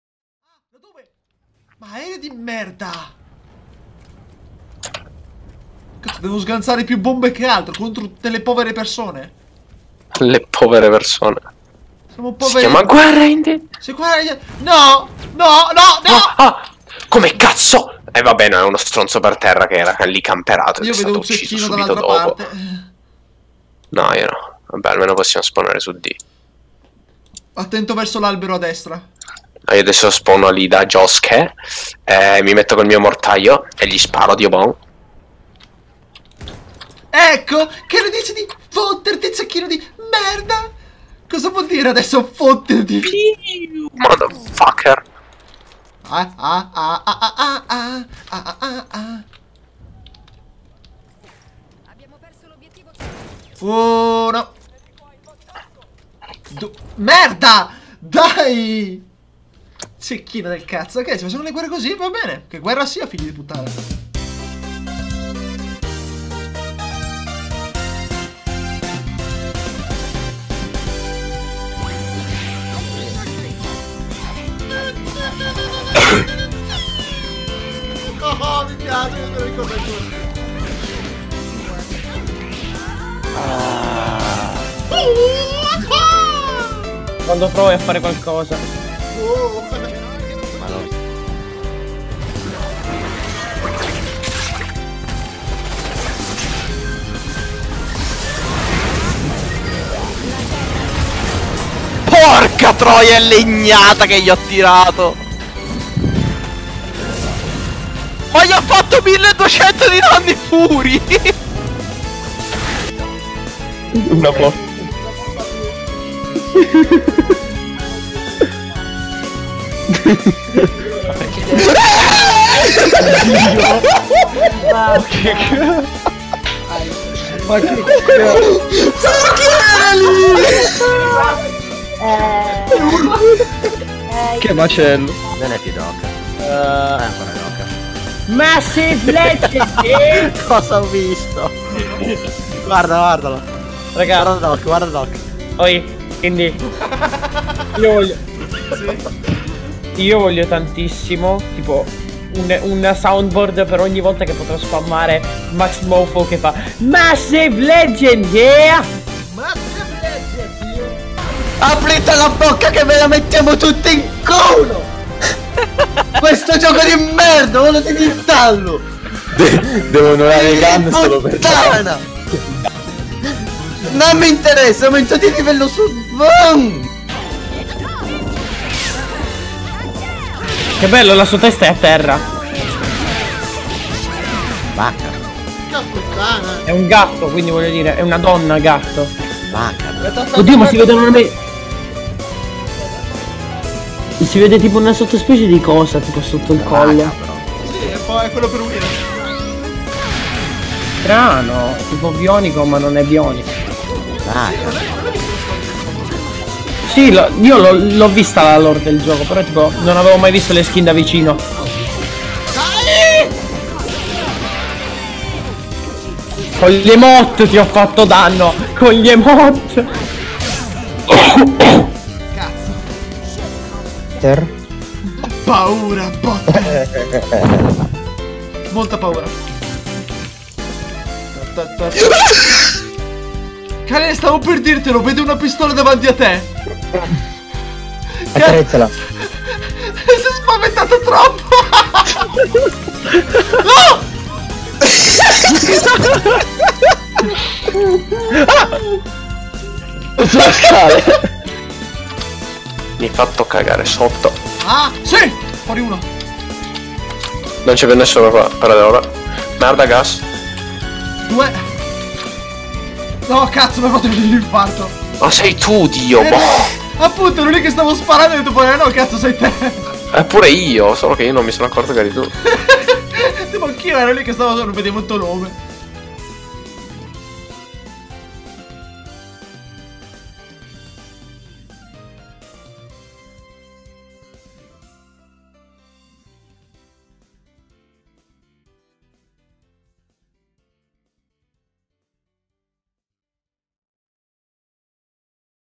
MOMENTI DA LUPO #1
Ecco una nuova serie stupida dove inserirò vari momenti strani e divertenti casuali su giochi che faccio insieme ad amici nei momenti di noia
Sperò vi piaccia lo stesso anche se dura poco